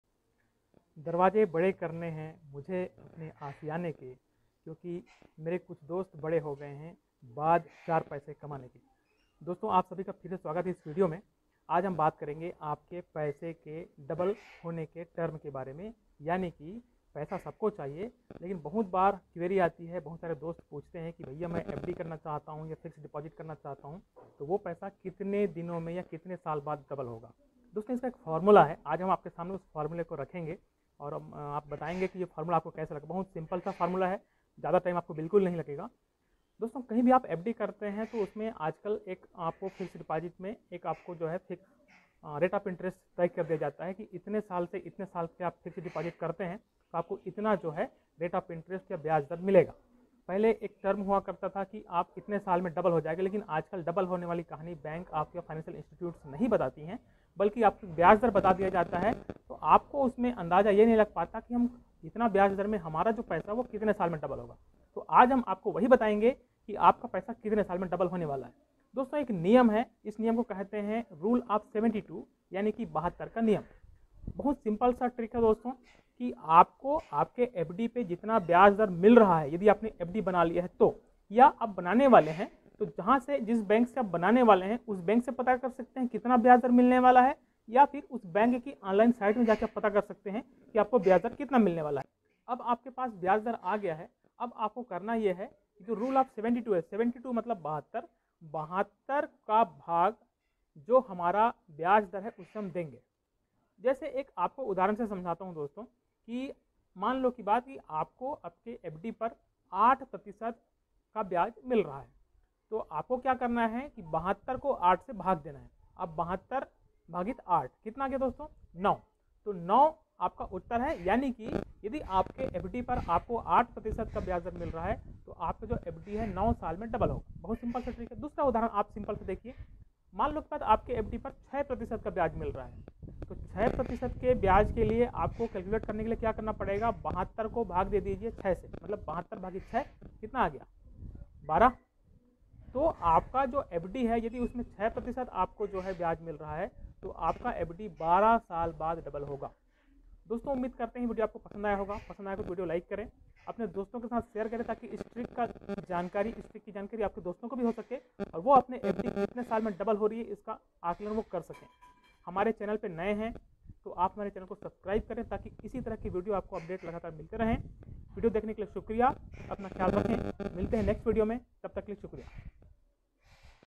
FD का पैसा कितने साल में दोगुना होता है | FD amount double in how many year | FD kitne sal me double
Formulla to calculate term for doubling of any fixed deposit

फिक्स्ड डिपॉजिट (मियादी जमा) कितने वर्ष में दोगुनी होगी, इसे बहुत ही आसान भाषा में सूत्र के माध्यम से समझाया गया है।